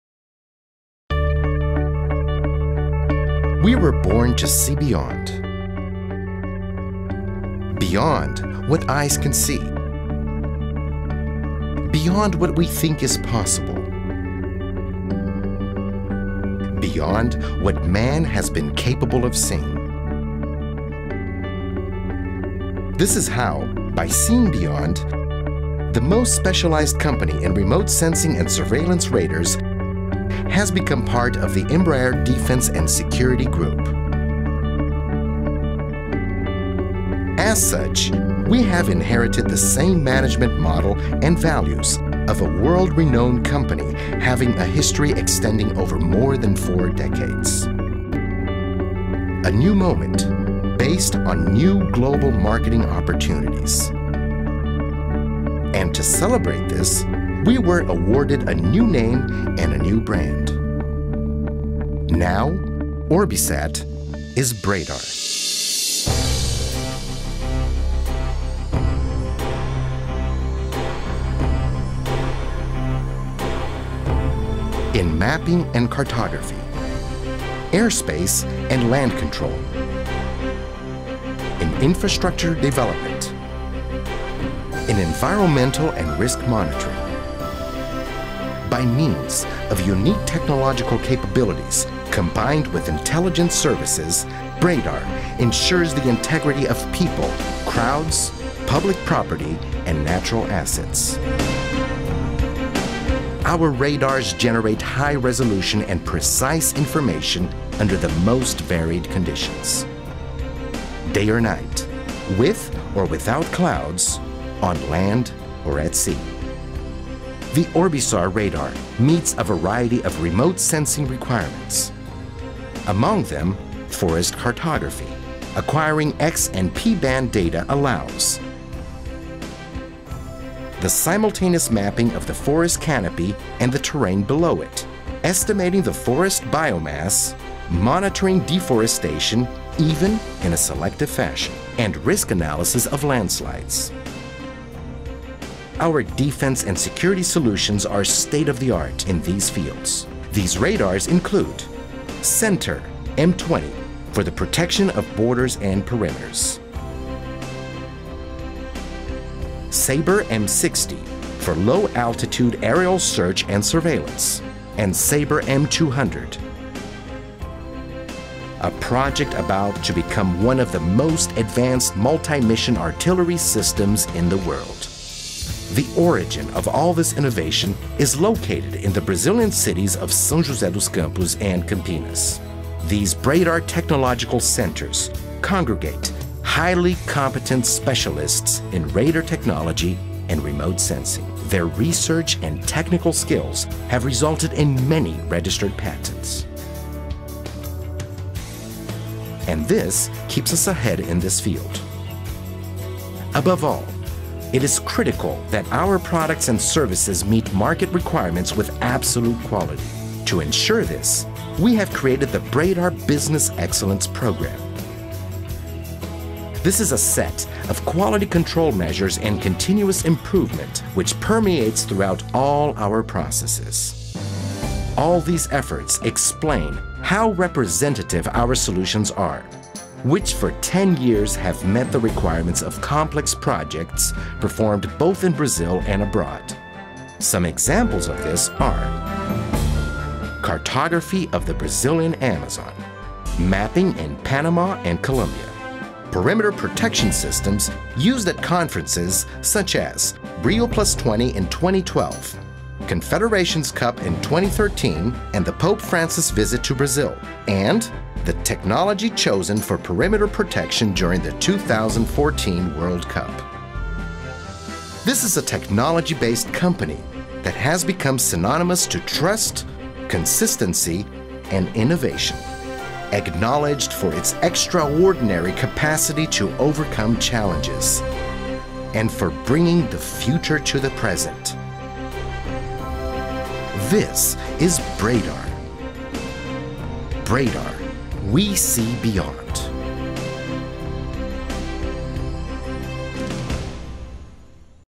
Institucional Video (English)
Bradar S.A. is a technology-based company, specialized in Synthetic Aperture Radars(SAR) and defense radars for aerial and terrestrial surveillance.